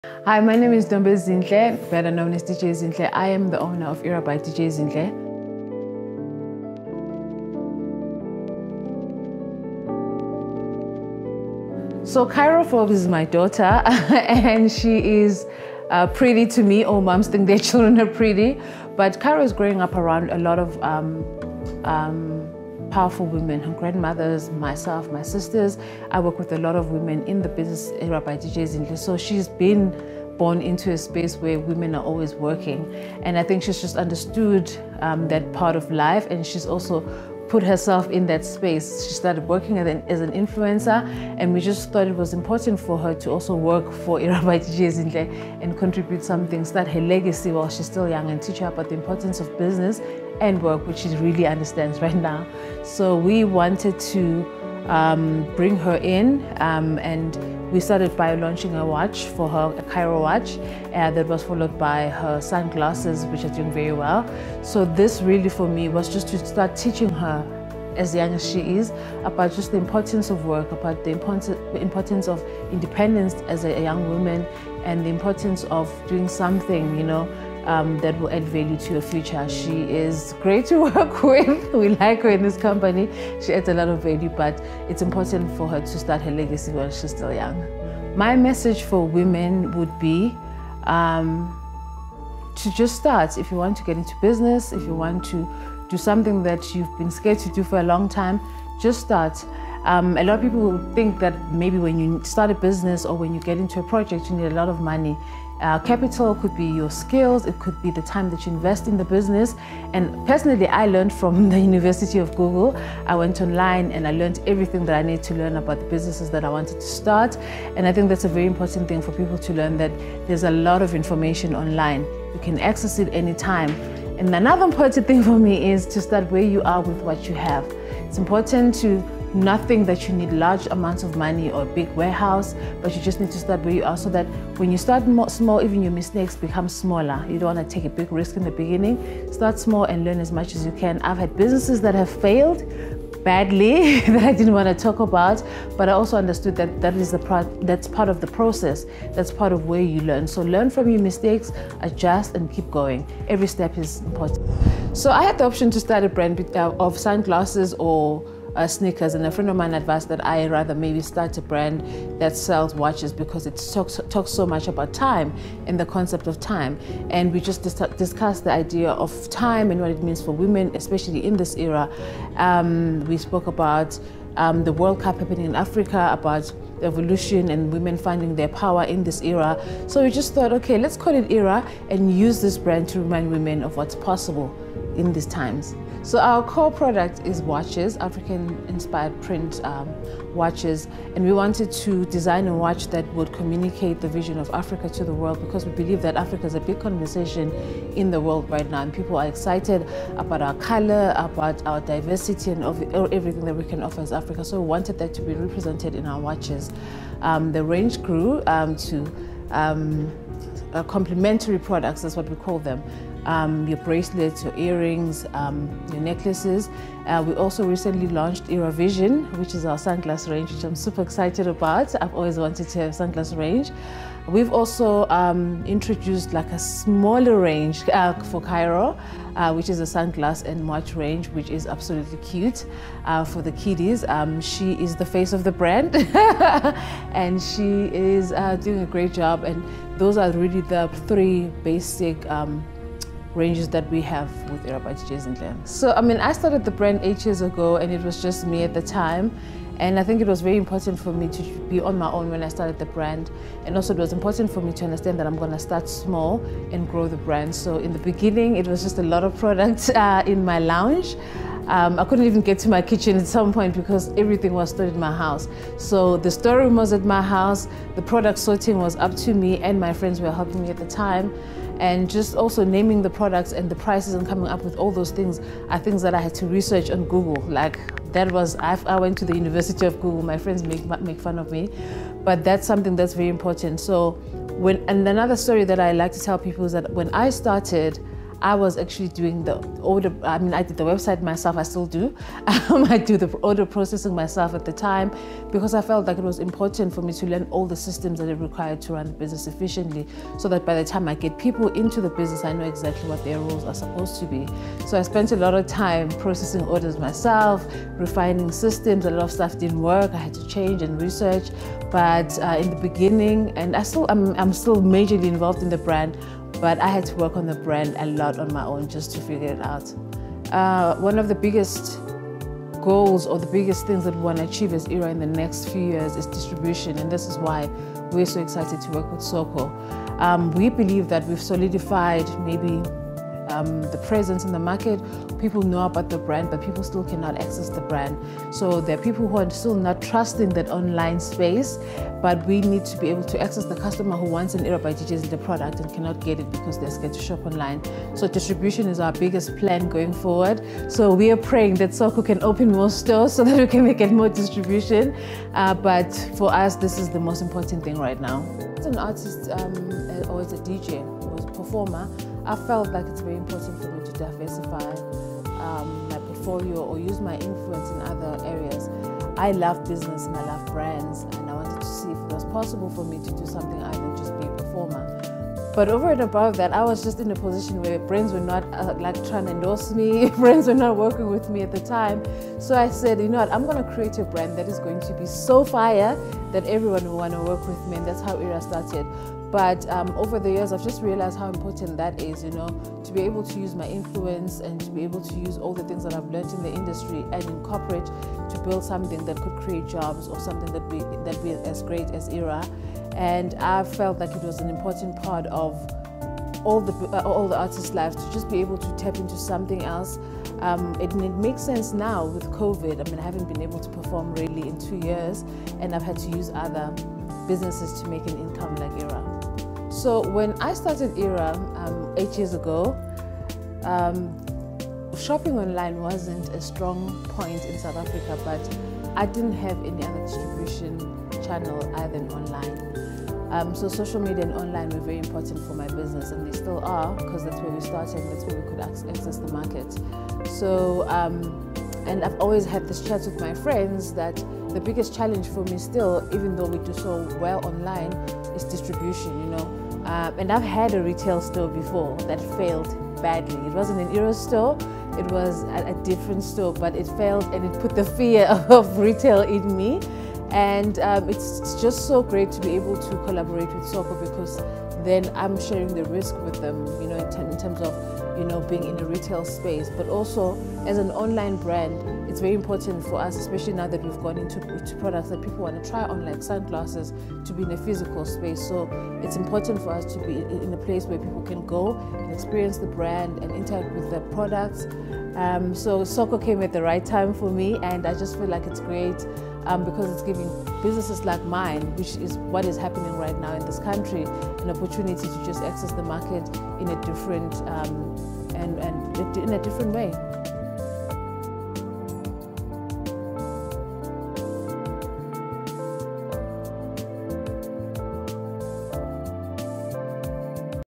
Richest Female Dj In Africa 2024 | How Dj Zinhle Makes her Millionaires in 2024 and her Businesses!
Dj Zinhle Examples how She made her First millionaire in Business. This video is short interview Documentary of Dj Zinhle about her journey to Success in Business.